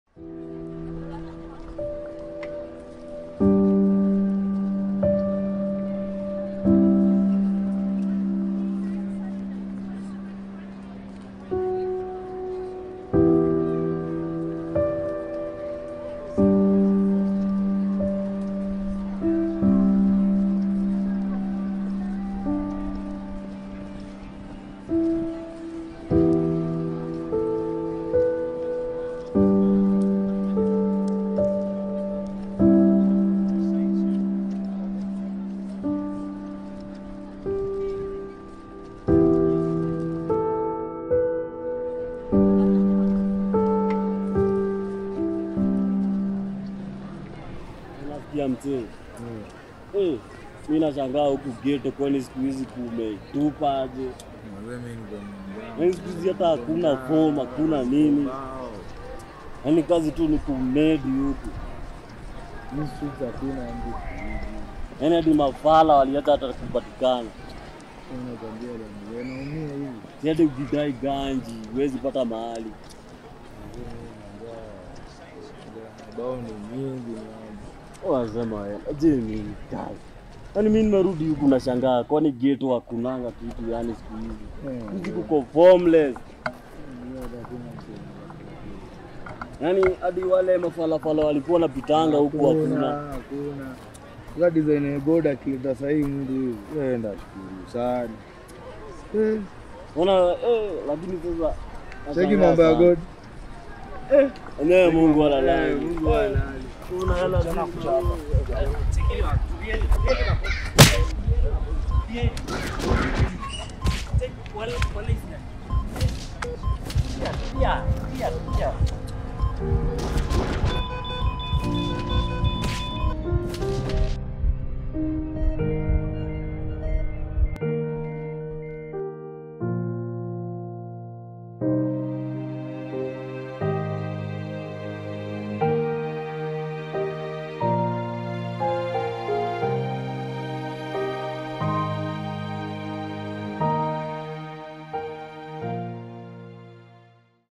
'ITS NOT WORTH IT'!!! Pillars of Art FT Jay.moh Pictures
With the rampant cases of robbery with violence and thefts, this short film is to sensitize on these cases and urge our fellow youths to stop it, people have lost lives due to this and as the video says its not worth it!